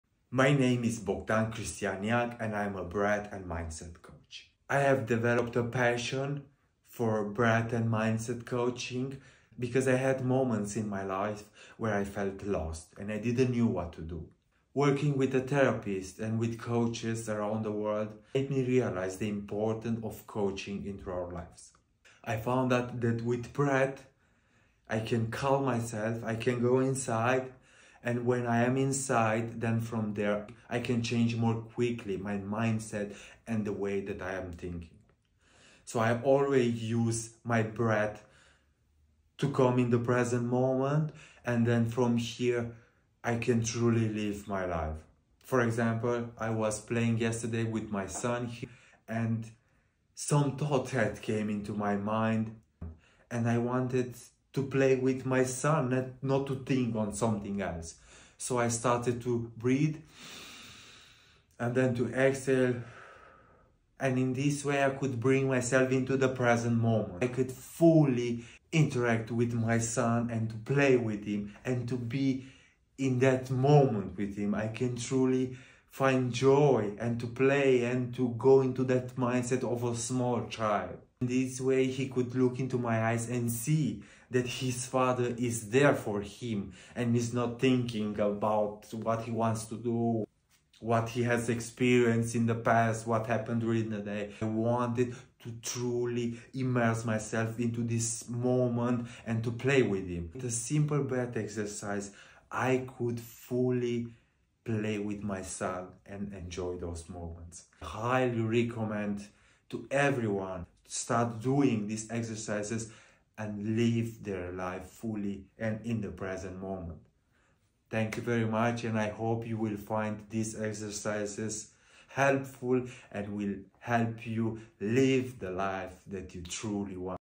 Welcome -I am Bogdan Cristian Neag, your Breath & Mindset Coach
Welcome to this brief introduction of myself, and why breathing has become a passion of mine. I hope you will discover the easy breathing techniques I am here to share with you in this short series of 5 introduction videos.